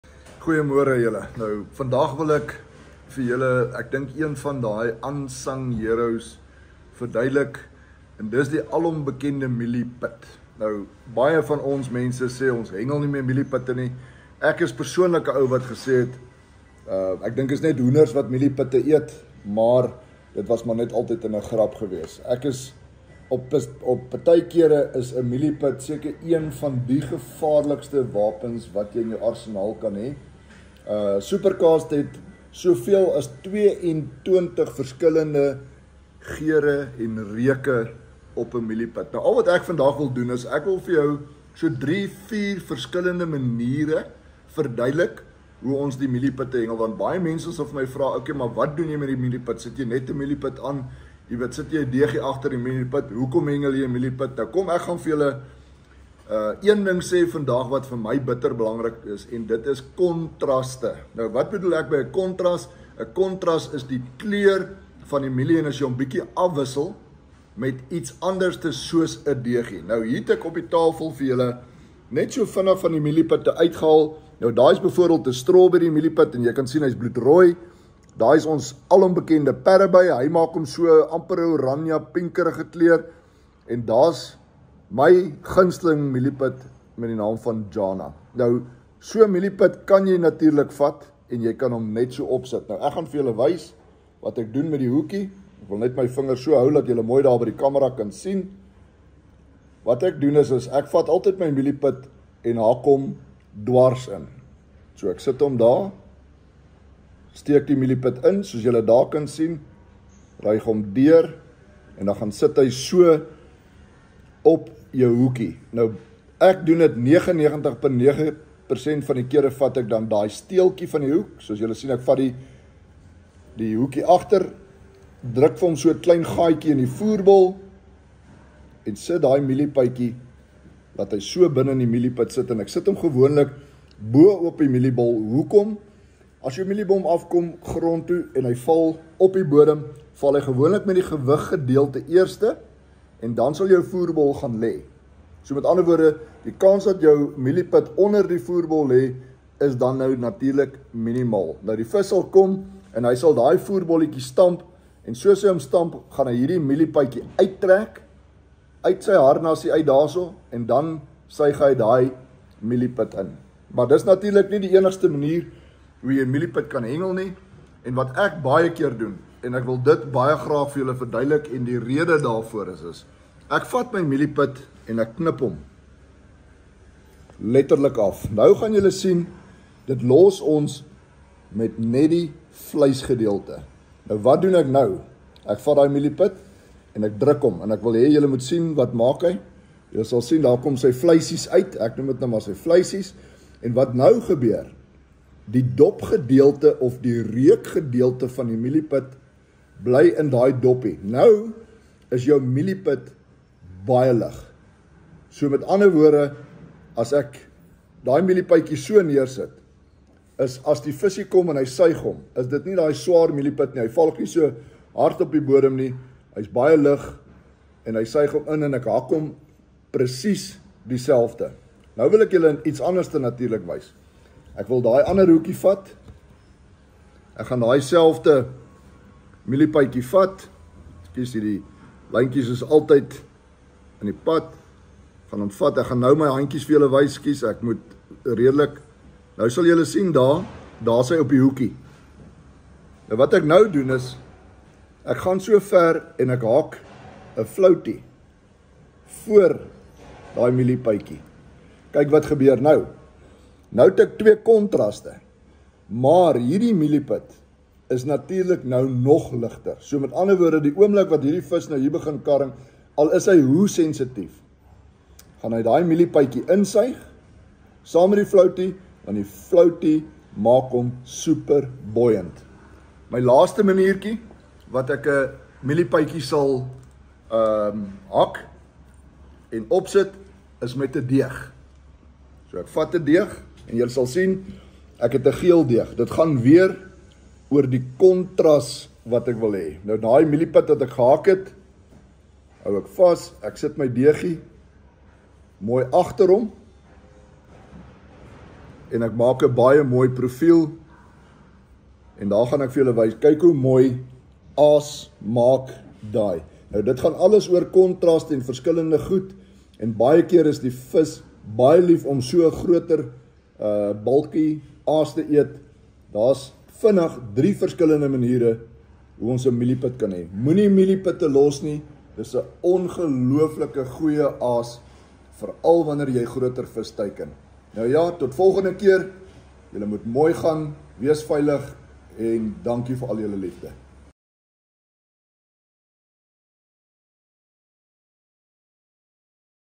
Mielie pit hengel wenke
Schalk gee ons n paar wenke spesifiek as jy met mielie pitte hengel.